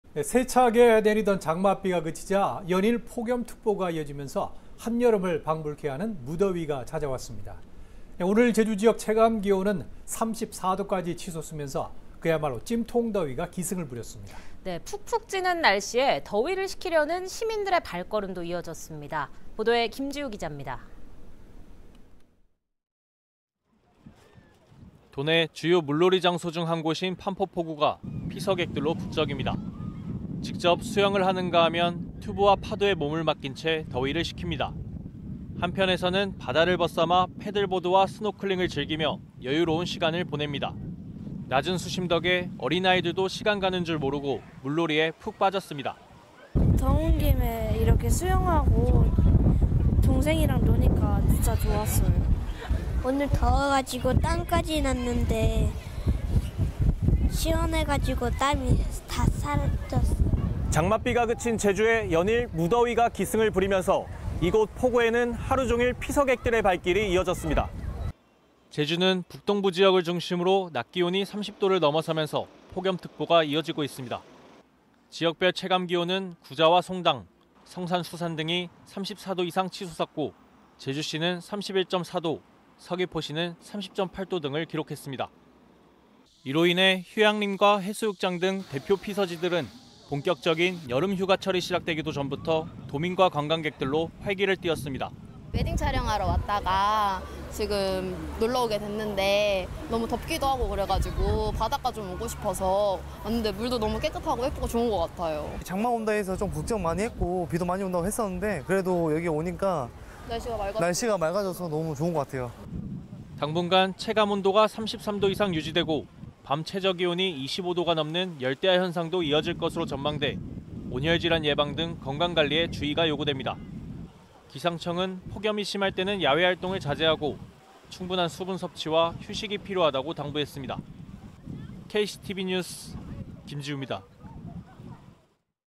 "34도 찜통더위 식히자"…피서객 '북적'
세차게 내리던 장맛비가 그치자 연일 폭염특보가 지속되고 있습니다.

오늘 제주지역 체감기온은 34도까지 치솟으면서 그야말로 찜통더위가 기승을 부렸습니다.

푹푹 찌는 날씨에 더위를 식히려는 시민들의 발걸음도 이어졌습니다.

도내 주요 물놀이 장소 중 한 곳인 판포포구가 피서객들로 북적입니다.

직접 수영을 하는가 하면 튜브와 파도에 몸을 맡긴 채 더위를 식힙니다.

한편에서는 바다를 벗삼아 패들보드와 스노클링을 즐기며 여유로운 시간을 보냅니다.

낮은 수심 덕에 어린아이들도 시간 가는 줄 모르고 물놀이에 푹 빠졌습니다.

[한지수 한지호 / 경기 김포]
"더운 김에 이렇게 수영하고 동생이랑 노니깐 진짜 좋았어요. 오늘 더워서 땀까지 났는데 시원해서 땀이 다 사라졌어요."

[김지우 기자]
"장맛비가 그친 제주에 연일 무더위가 기승을 부리면서 이곳 포구에는 하루 종일 피서객들의 발길이 이어졌습니다."

제주는 북동부 지역을 중심으로 낮 기온이 30도를 넘어서면서 폭염특보가 이어지고 있습니다.

지역별 체감기온은 구좌와 송당, 성산 수산 등이 34도 이상 치솟았고 제주시는 31.4도, 서귀포시는 30.8도 등을 기록했습니다.

이로 인해 휴양림과 해수욕장 등 대표 피서지들은 본격적인 여름 휴가철이 시작되기도 전부터 도민과 관광객들로 활기를 띄었습니다.

[김지수, 선호 / 경북 포항]
"웨딩 촬영하러 왔다가 지금 놀러 오게 됐는데 너무 덥기도 하고 그래서 바닷가 오고 싶어서 왔는데 물도 너무 깨끗하고 예쁘고 좋은 것 같아요. 장마 온다 해서 걱정 많이 했고 비도 많이 온다 했었는데 그래도 여기 오니깐 날씨가 맑아져서 너무 좋은 것 같아요."

당분간 체감온도가 33도 이상 유지되고 밤 최저기온이 25도가 넘는 열대야 현상도 이어질 것으로 전망돼 온열질환 예방 등 건강관리에 주의가 요구됩니다.

기상청은 폭염이 심할 때는 야외활동을 자제하고 충분한 수분 섭취와 휴식이 필요하다가 당부했습니다.

(영상취재 김승철)